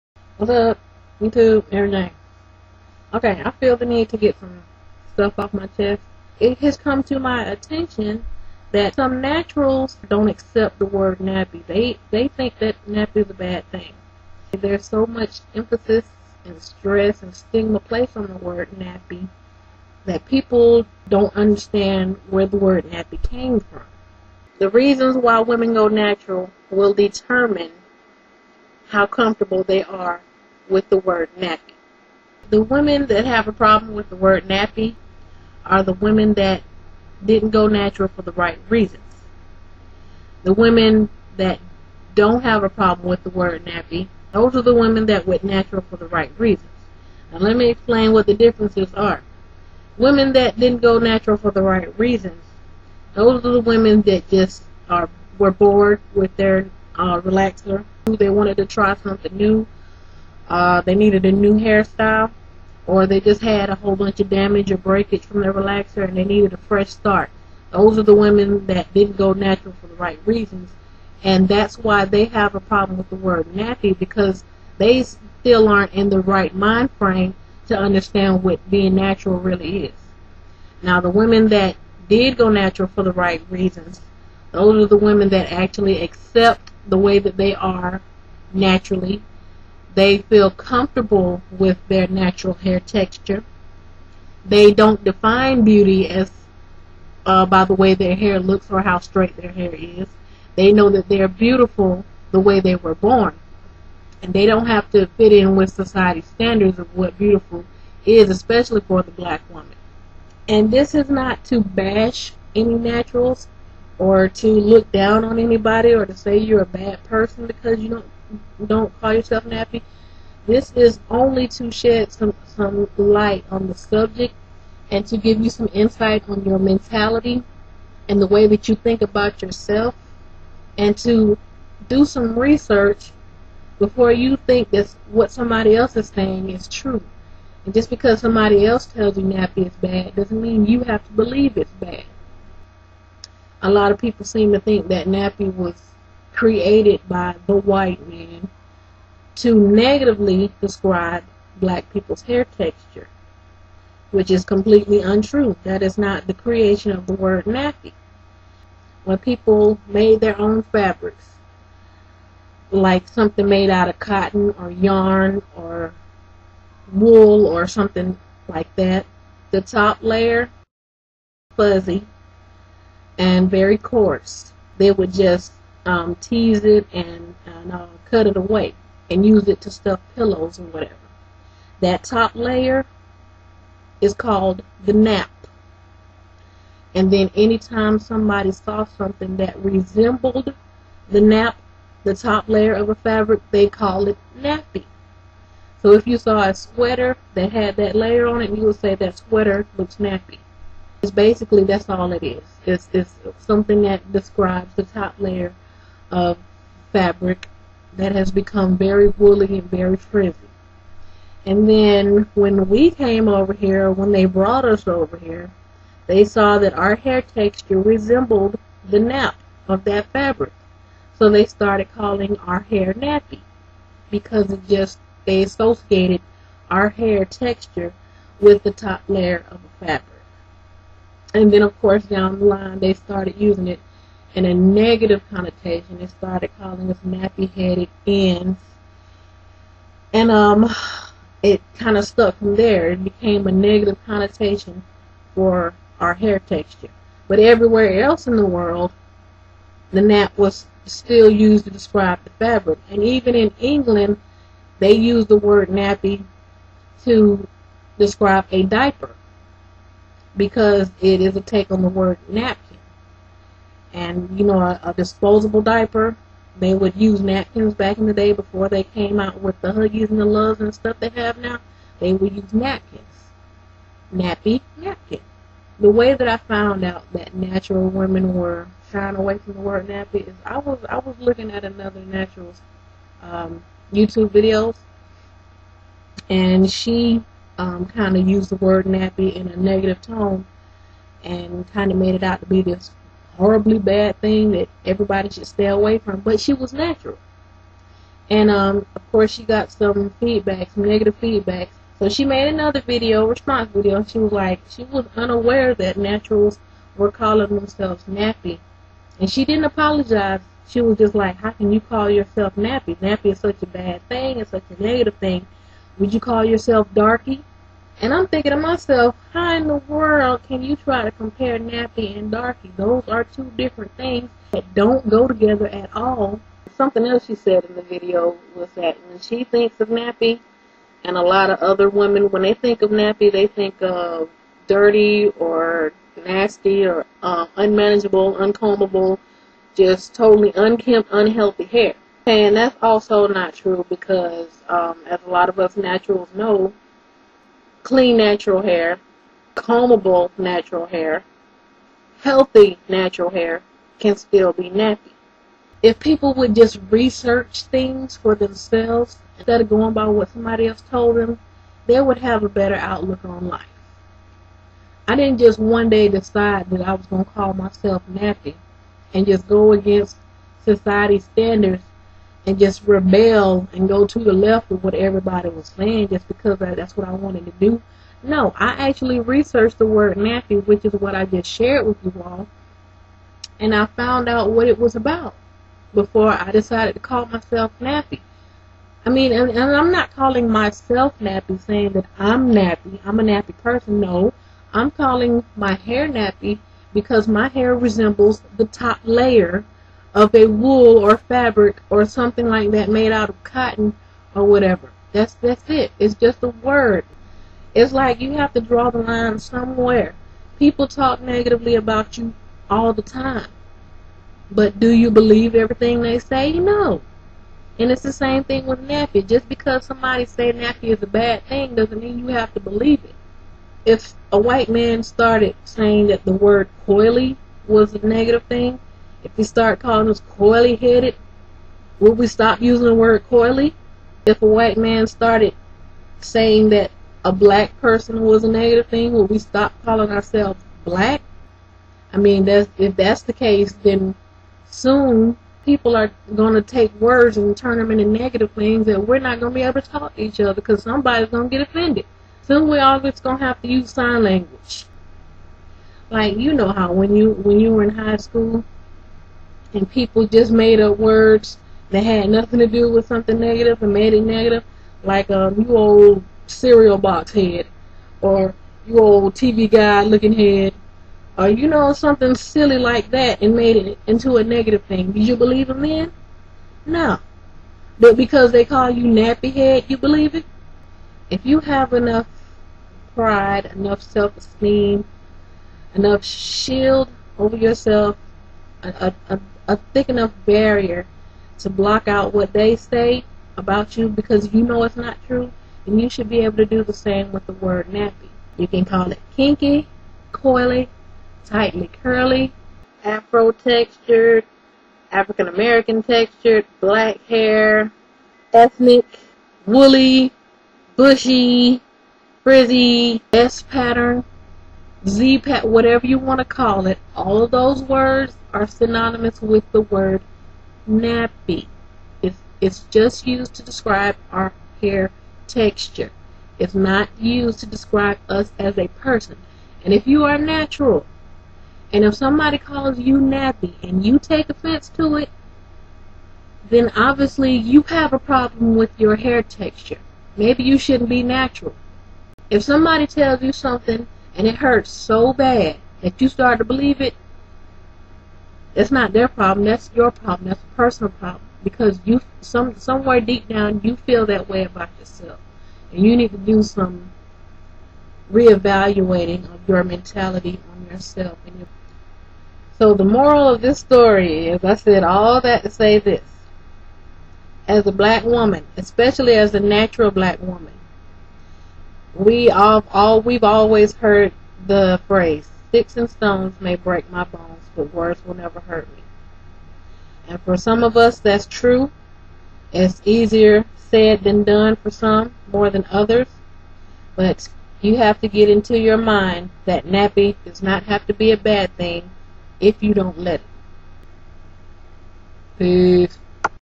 NAPPY?!?!
I discuss the mentality that society puts on the dreaded n-word. No, not that one, NAPPY! Specifically, I discuss the origin of the word, the stigma other naturals place on the word, and a different way to look at how it makes us feel.